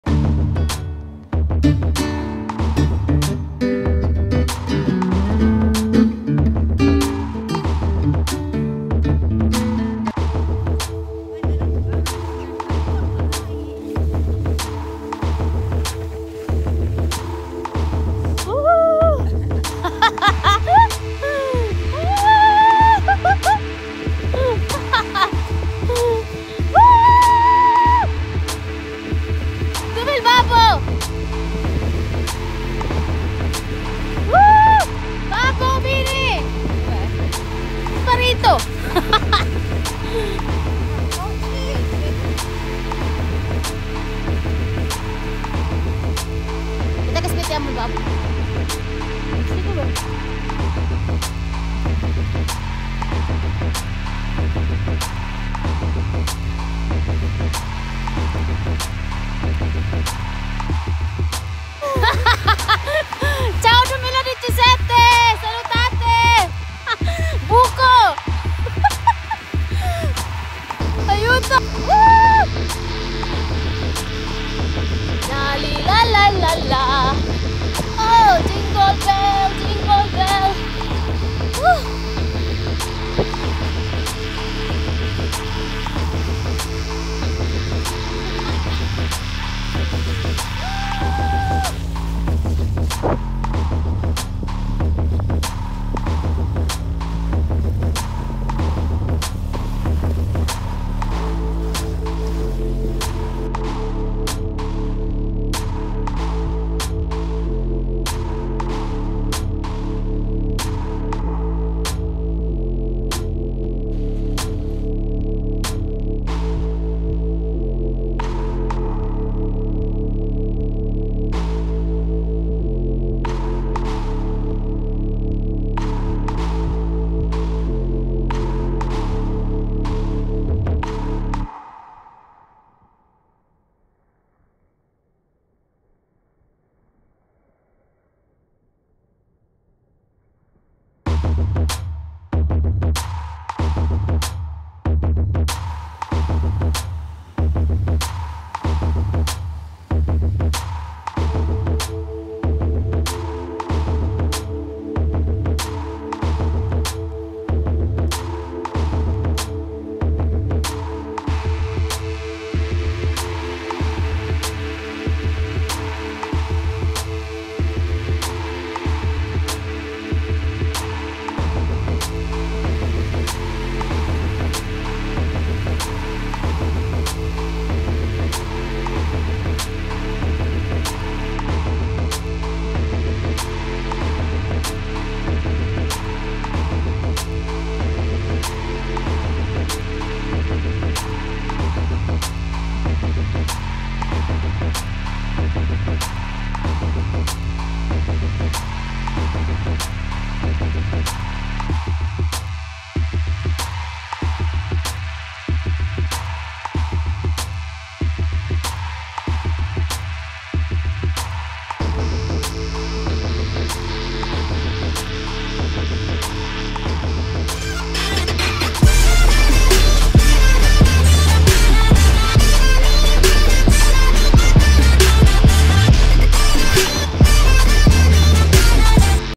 MY LIFE IN ITALY: SLEDDING ON THE SNOW/SUPER FUN DAY/LAUGHING IS THE BEST MEDECINE😂❤️
hi guys this video is about hanging out and having good times with fambam on snow